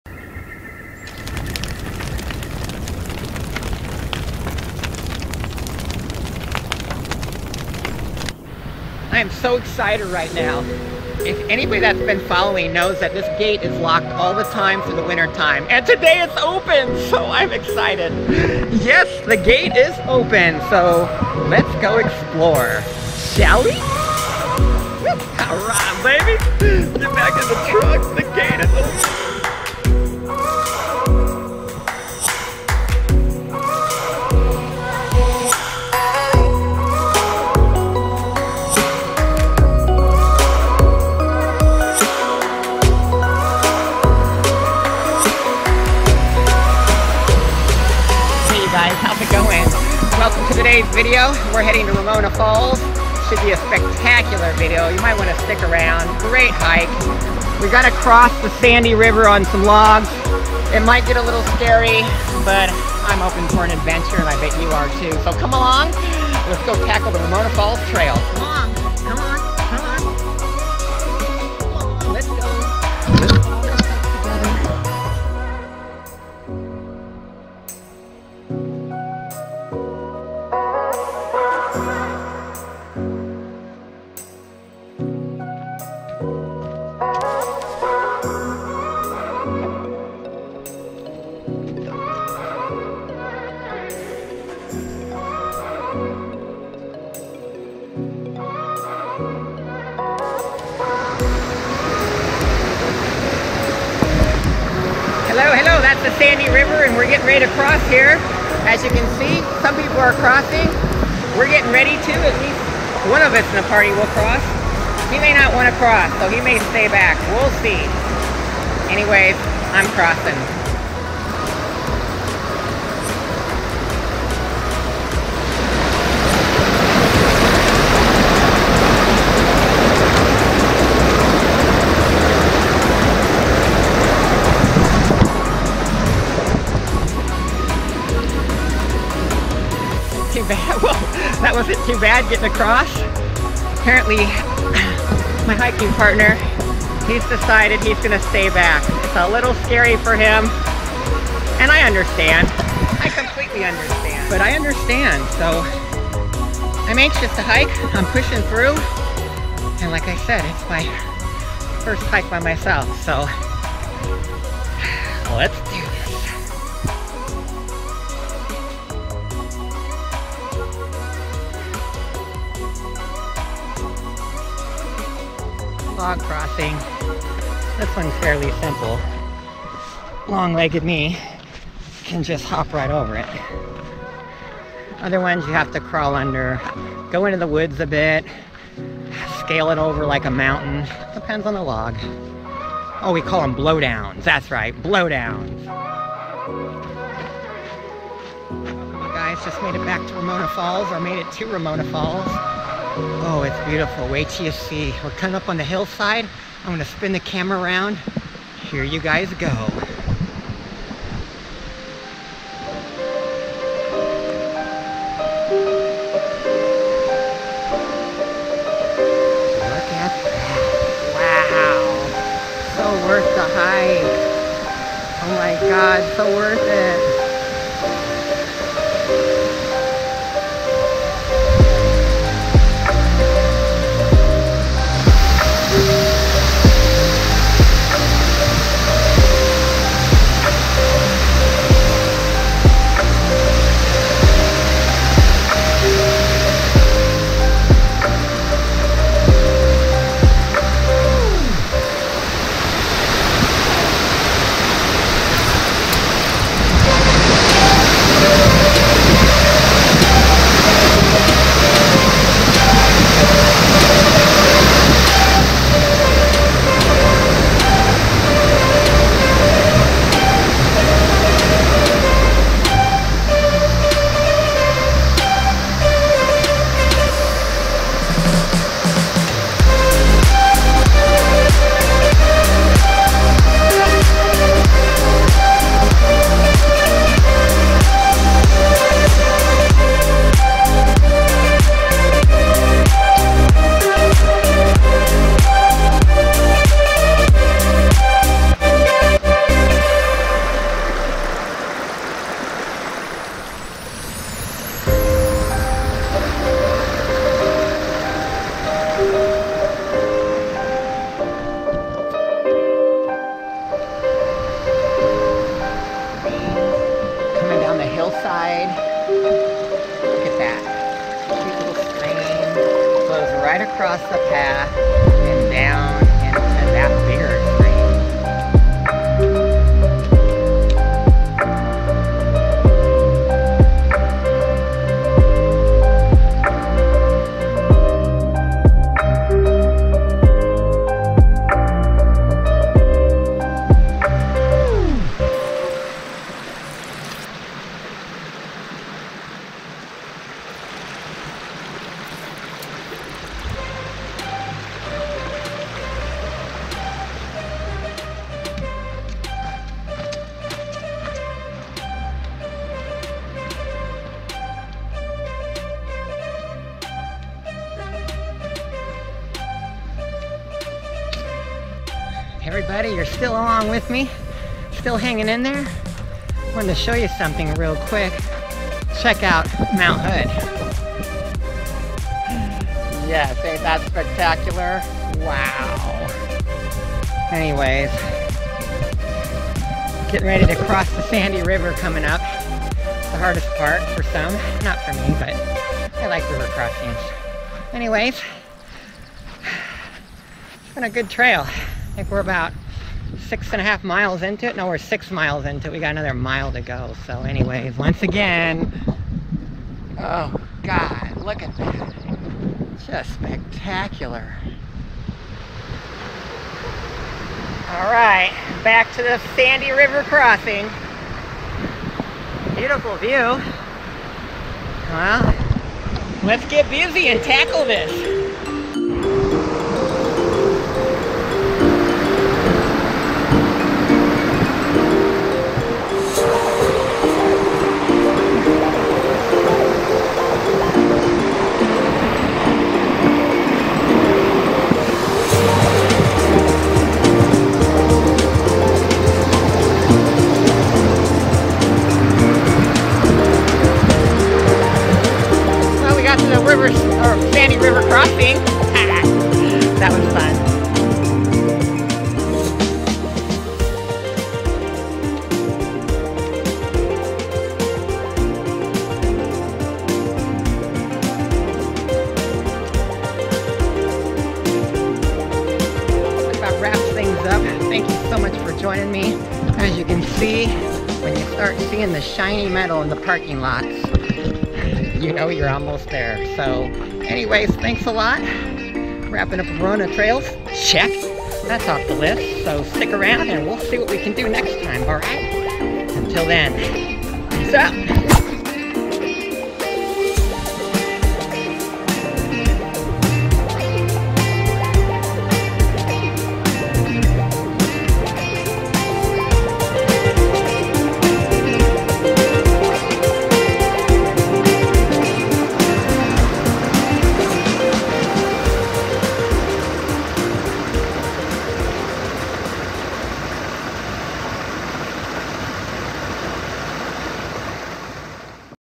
Solo hiking Ramona Falls
Hiking Ramona Falls solo after a Fast moving river crossing cautions  partner to  turn back. i push on to share the Beautiful falls with you. Come share my hike